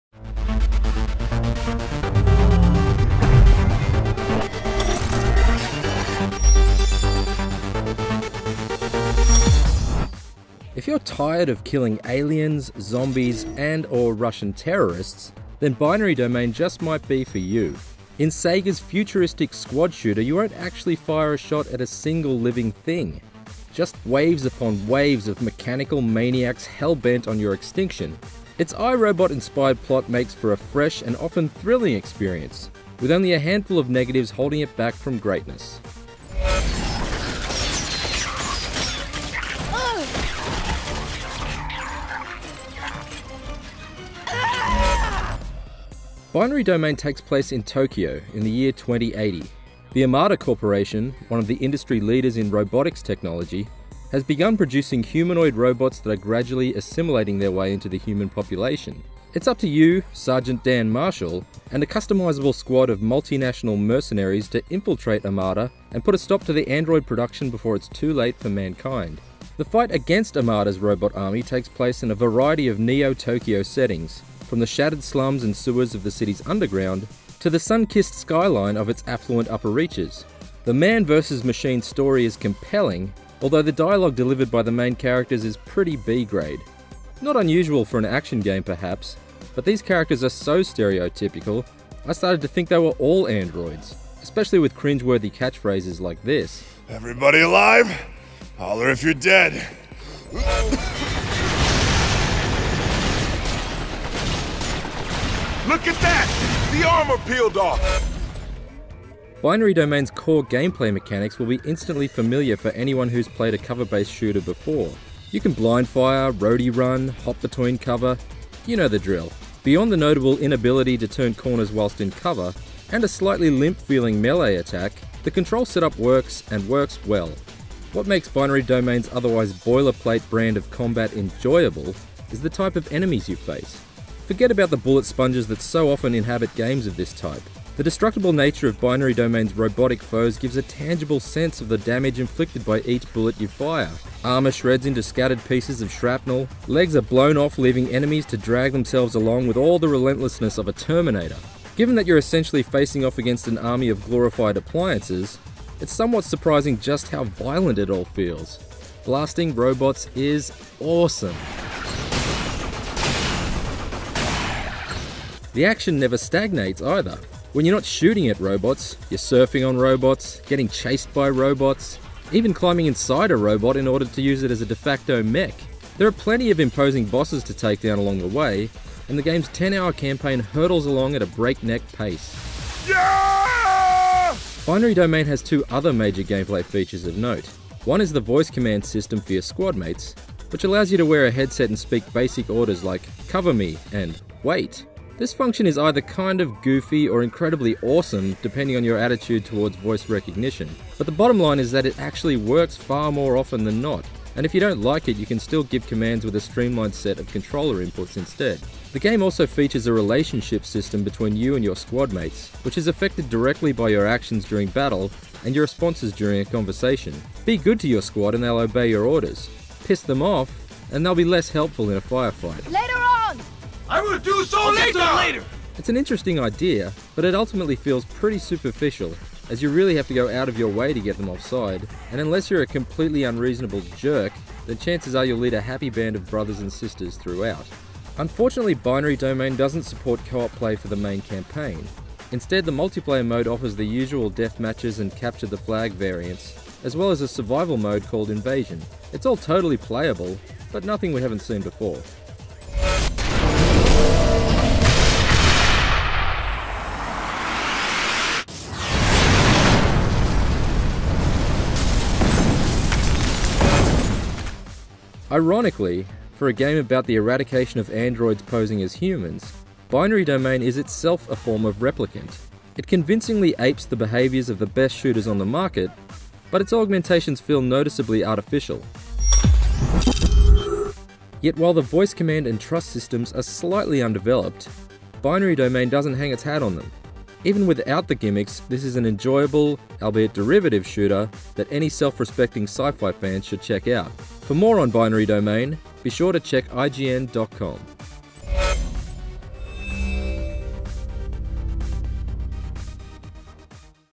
IGN Reviews - Binary Domain - Game Review
IGN Editor Tristan Ogilvie reviews SEGAs new third person, cover based shooter Binary Domain. Will SEGAs new IP have you blasting Rage Against the Machine as you mow down enemy robots or is this a title you should skip?

Release Date: February 28, 2012
Platforms: PS3, Xbox 360
Rated: M
Genre: Action
Publisher: SEGA
Developer: SEGA CS